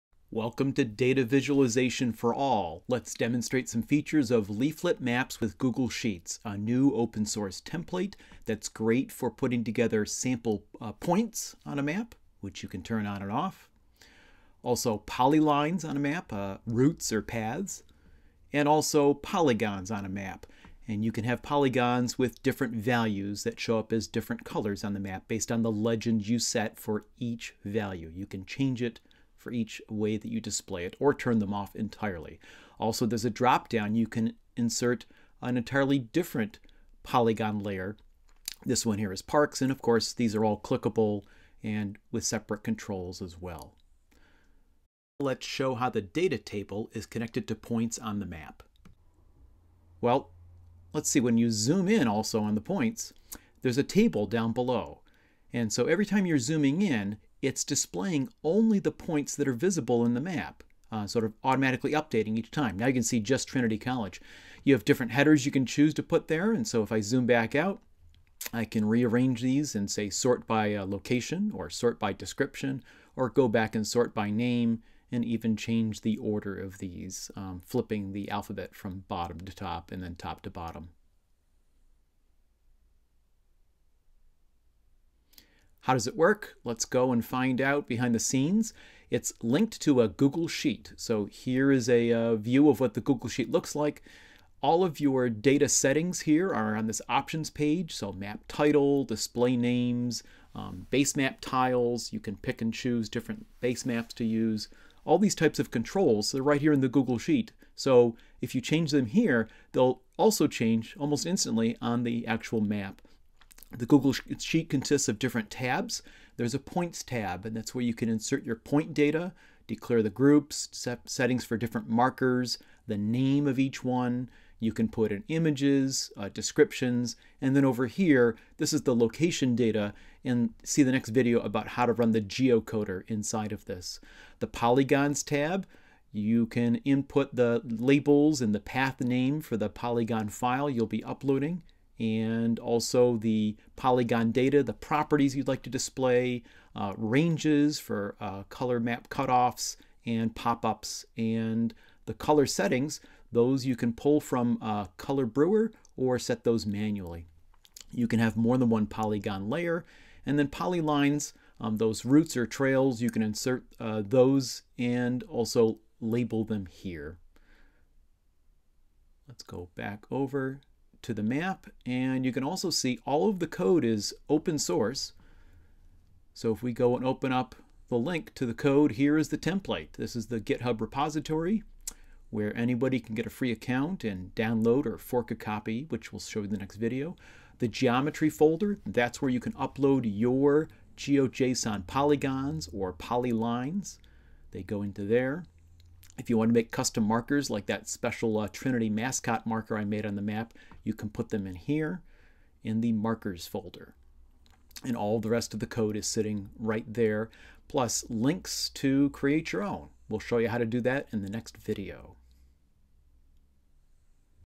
Leaflet Maps with Google Sheets - features
created for Data Visualization for All, a free book and online course, -- needs captions